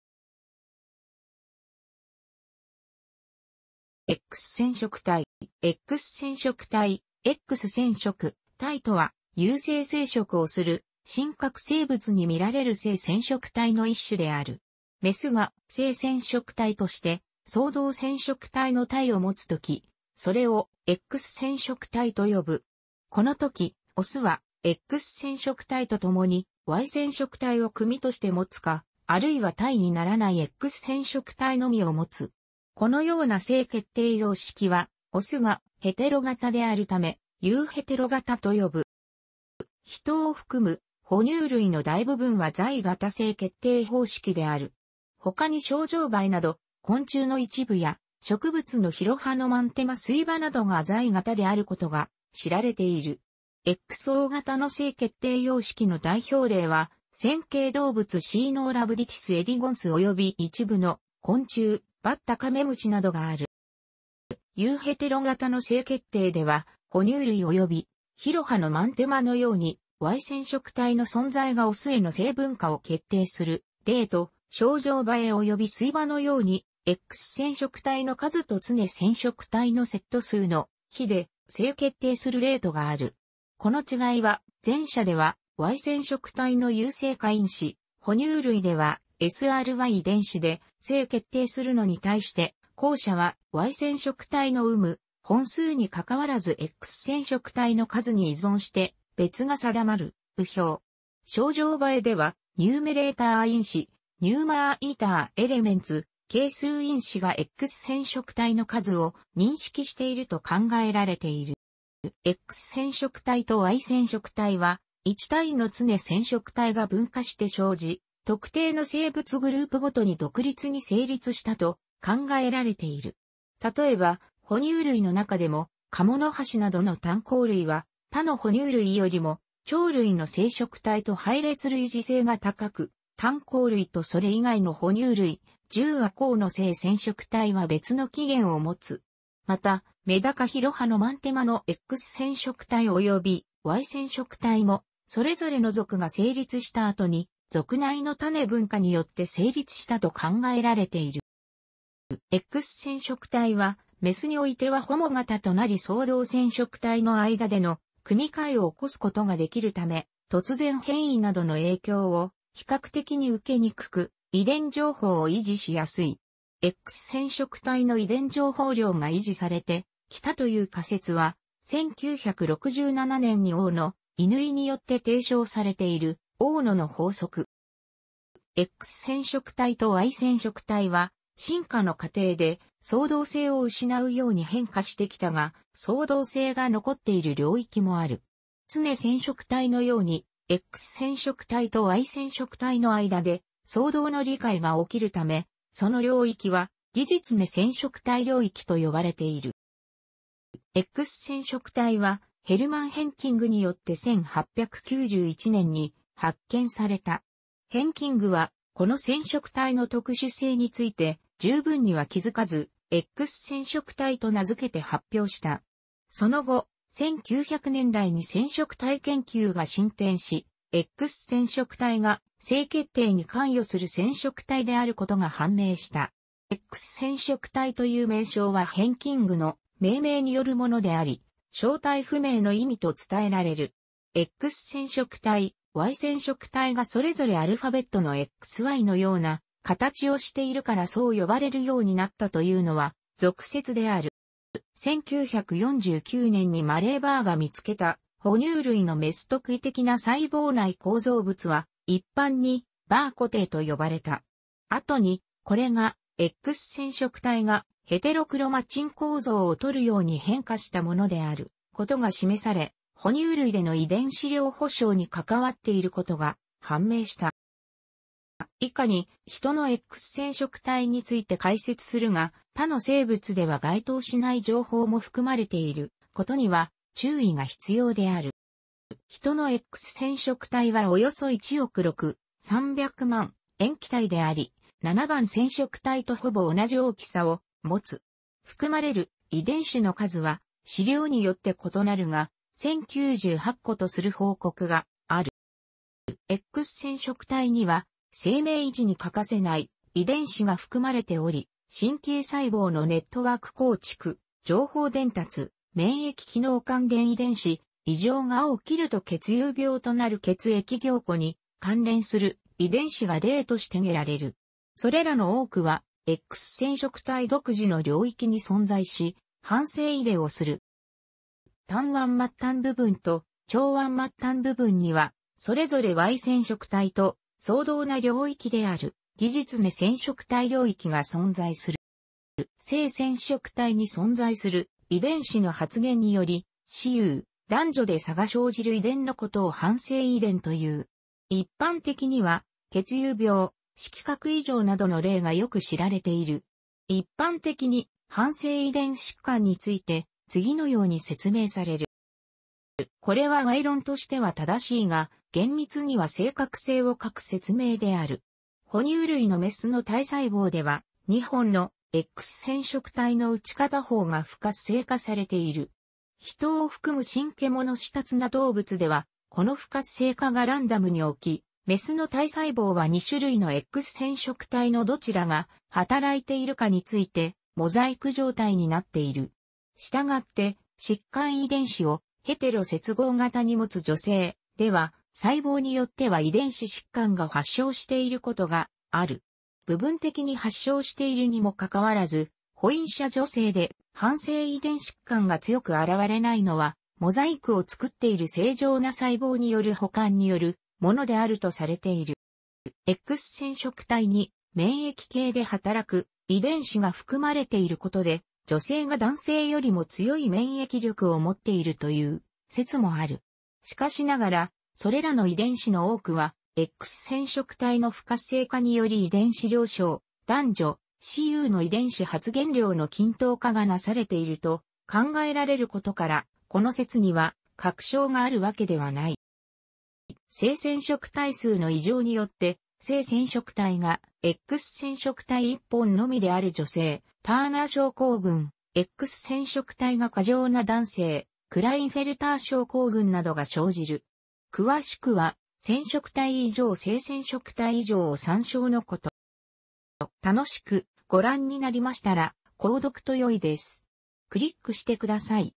X染色体
X染色体（エックスせんしょくたい）とは有性生殖をする真核生物にみられる性染色体の一種である。雌が性染色体として相同染色体の対を持つとき、それをX染色体と呼ぶ。このとき、雄はX染色体と共にY染色体を組として持つか(XY型)、あるいは対にならないX染色体のみを持つ(XO型)。このような性決定様式は雄がヘテロ型であるため「雄ヘテロ型」と呼ぶ。

ヒトを含む哺乳類の大部分はXY型性決定方式である。他にショウジョウバエなど昆虫の一部や植物のヒロハノマンテマ・スイバなどがXY型であることが知られている。XO型の性決定様式の代表例は、線形動物"Caenorhabditis elegans"および一部の昆虫（バッタ・カメムシ）などがある。

雄ヘテロ型の性決定では、哺乳類およびヒロハノマンテマのようにY染色体の存在が雄への性分化を決定する例と、ショウジョウバエおよびスイバのようにX染色体の数と常染色体のセット数の比率(X/A)で性決定する例とがある。この違いは、前者ではY染色体の雄性化因子（哺乳類では"SRY"遺伝子）で性決定するのに対して、後者はY染色体の有無・本数に関わらずX染色体の数に依存して性別が定まる（右表）。ショウジョウバエでは、ニューメレーター因子（numerator elements, 計数因子）がX染色体の数を認識していると考えられている。

X染色体とY染色体は、1対の常染色体が分化して生じ、特定の生物グループごとに独立に成立したと考えられている。例えば、哺乳類の中でもカモノハシなどの単孔類は他の哺乳類よりも鳥類の性染色体と配列類似性が高く、単孔類とそれ以外の哺乳類（獣亜綱）の性染色体は別の起原を持つ。また、メダカ・ヒロハノマンテマのX染色体およびY染色体も、それぞれの属が成立した後に、属内の種分化によって成立したと考えられている。

X染色体は、雌においてはホモ型となり相同染色体の間での組換えを起こすことができるため、突然変異などの影響を比較的に受けにくく、遺伝情報を維持しやすい。X染色体の遺伝情報量が維持されてきたという仮説は、1967年に大野乾によって提唱されている（大野の法則）。

X染色体とY染色体は、進化の過程で相同性を失うように変化してきたが、相同性が残っている領域もある。常染色体のようにX染色体とY染色体の間で相同乗り換えが起きるため、その領域は擬似常染色体領域(PAR, pseudoautosomal region)と呼ばれている。

X染色体はヘルマン・ヘンキングによって1891年に発見された。ヘンキングは、この染色体の特殊性について充分には気づかず「X染色体」と名付けて発表した。その後、1900年代に染色体研究が進展し、X染色体が性決定に関与する染色体であることが判明した。X染色体という名称はヘンキングの命名によるものであり「正体不明」の意味と伝えられる。X染色体・Y染色体がそれぞれアルファベットのX・Yのような形をしているからそう呼ばれるようになったというのは俗説である。

1949年にマレー・バーが見つけた哺乳類の雌特異的な細胞内構造物は、一般にバー小体と呼ばれた。後に、これがX染色体がヘテロクロマチン構造をとるように変化したものであることが示され、哺乳類での遺伝子量補償に関わっていることが判明した。

以下にヒトのX染色体について解説するが、他の生物では該当しない情報も含まれていることには注意が必要である。

ヒトのX染色体はおよそ1億6,300万塩基対であり、7番染色体とほぼ同じ大きさを持つ。含まれる遺伝子の数は、資料によって異なるが、1,098個とする報告がある。

X染色体には生命維持に欠かせない遺伝子が含まれており、神経細胞のネットワーク構築・情報伝達、免疫機能関連遺伝子、（異常が起きると血友病となる）血液凝固に関連する遺伝子が例として挙げられる。それらの多くはX染色体独自の領域に存在し、伴性遺伝をする。

短腕末端部分と長腕末端部分にはそれぞれY染色体と相同な領域である擬似常染色体領域が存在する。

性染色体に存在する遺伝子の発現により、雌雄・男女で差が生じる遺伝のことを伴性遺伝という。一般的には血友病・色覚異常などの例が良く知られている。一般的に伴性遺伝疾患について次のように説明される。

これは概論としては正しいが、厳密には正確性を欠く説明である。哺乳類の雌の体細胞では、2本のX染色体のうち片方が不活性化されている。ヒトを含む真獣下綱動物では、この不活性化が...